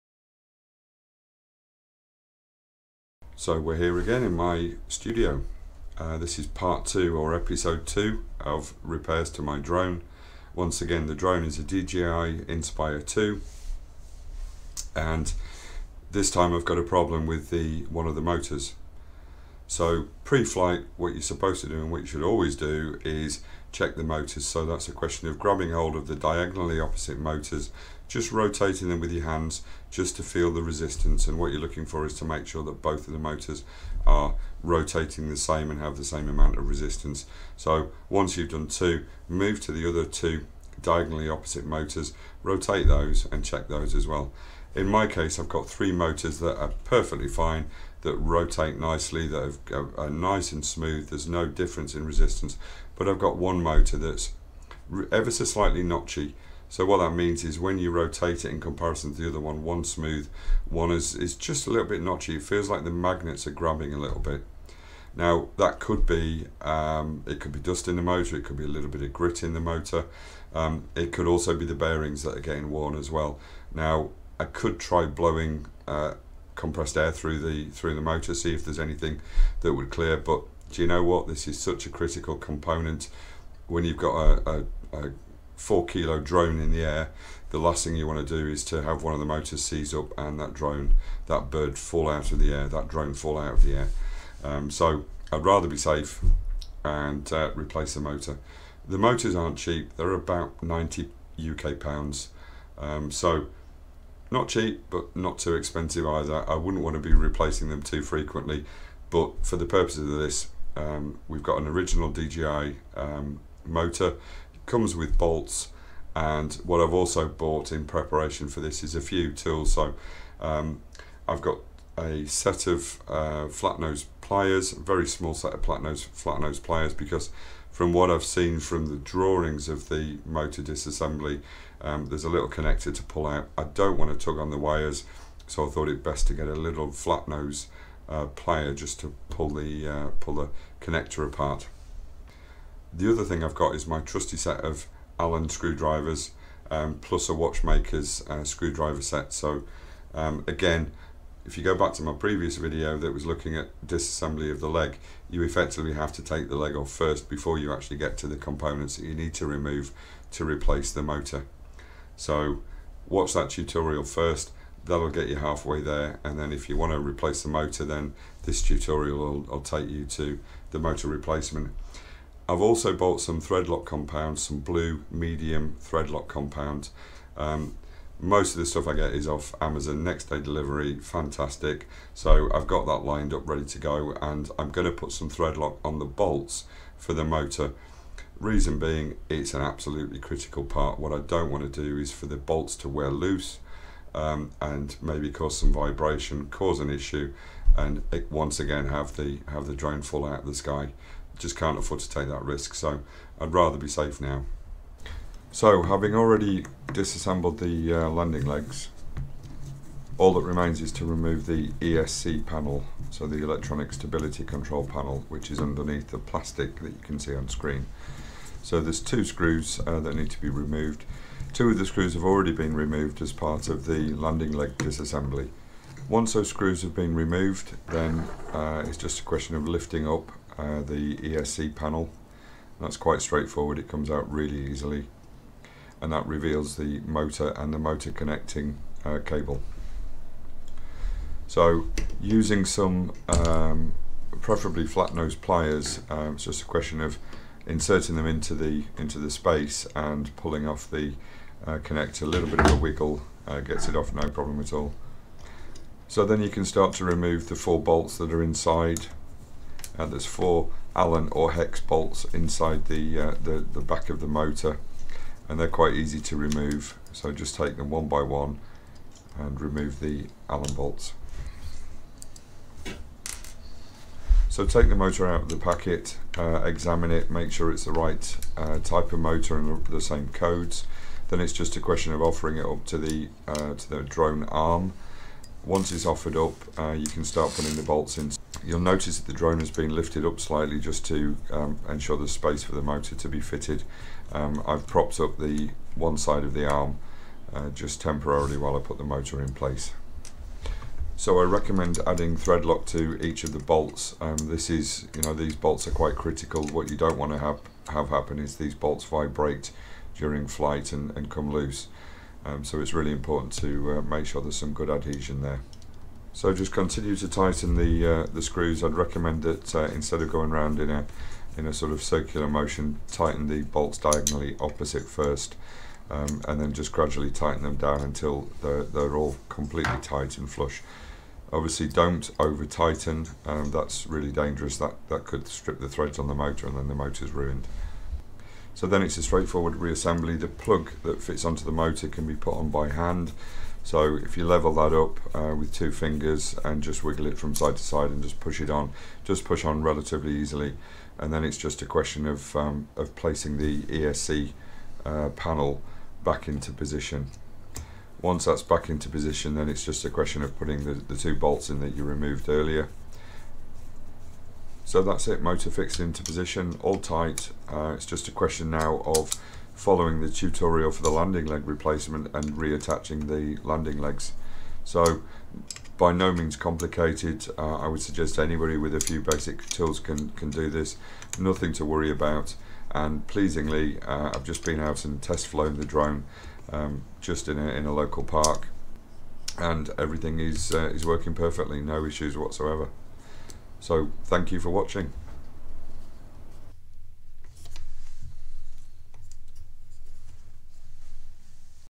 DJI Inspire 2 Repair - Motor Replacement
Tutorial showing how to replace a motor on a DJI Inspire 2

Watch my previous landing leg replacement tutorial first, as this gets you half way to a motor replacement.

Tip: The propeller mount torx bolt head stripped when trying to remove from the old motor, so ensure you have a new replacement mount for the new motor - probably a good idea anyway to replace it.